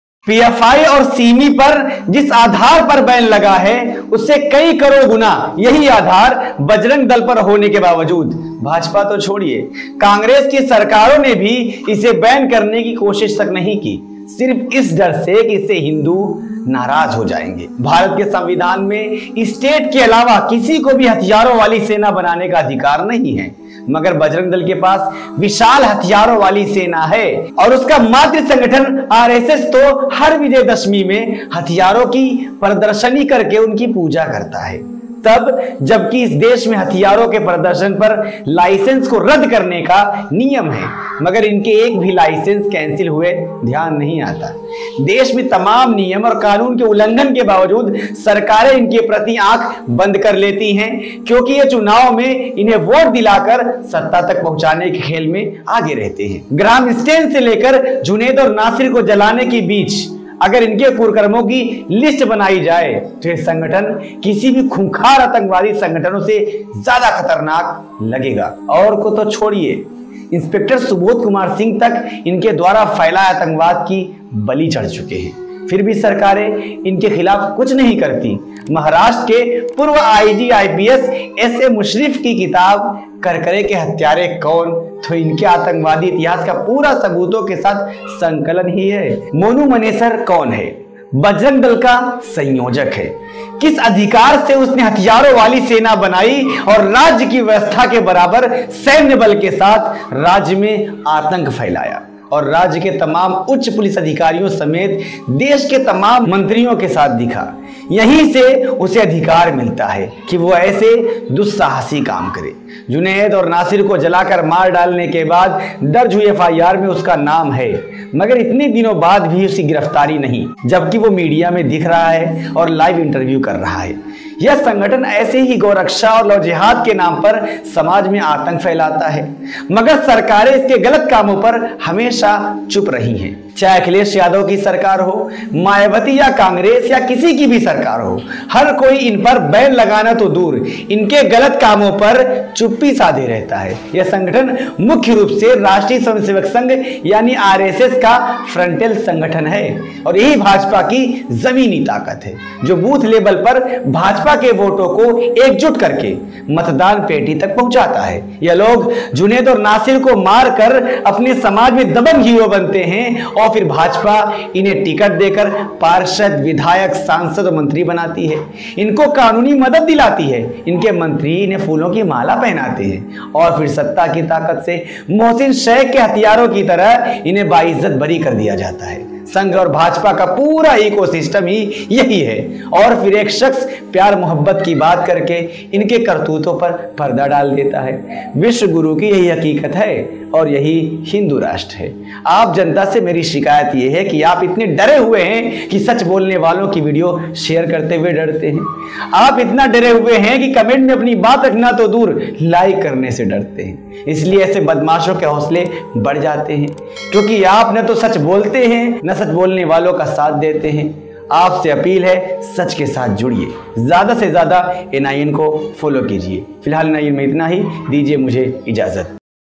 क्या नासिर-जुनैद को मध्यम वर्ग ने मारा है? Has the middle class killed Nasir-Junaid?
आतंकवादी संग़ठन बजरंग दल को सरकार दे रही संरक्षण